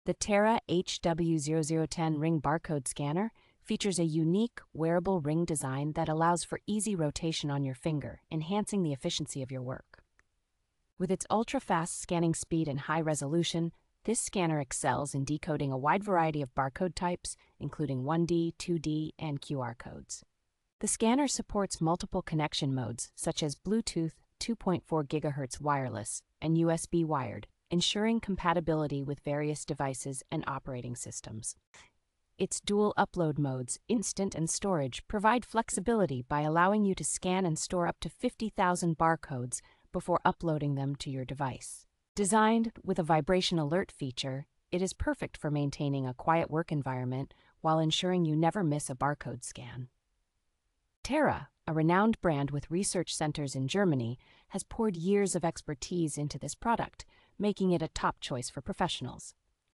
REVIEW (2024): HW0010 Ring Barcode Scanner. ESSENTIAL details.
The Tera HW0010 Ring Barcode Scanner features a unique wearable ring design that allows for easy rotation on your finger, enhancing the efficiency of your work.

With its ultra-fast scanning speed and high resolution, this scanner excels in decoding a wide variety of barcode types, including 1D, 2D, and QR codes.

The scanner supports multiple connection modes, such as Bluetooth, 2.4 gigahertz wireless, and USB wired, ensuring compatibility with various devices and operating systems.

Its dual upload modes, instant and storage, provide flexibility by allowing you to scan and store up to fifty thousand barcodes before uploading them to your device.

Designed with a vibration alert feature, it is perfect for maintaining a quiet work environment while ensuring you never miss a barcode scan.

Tera, a renowned brand with research centers in Germany, has poured years of expertise into this product, making it a top choice for professionals.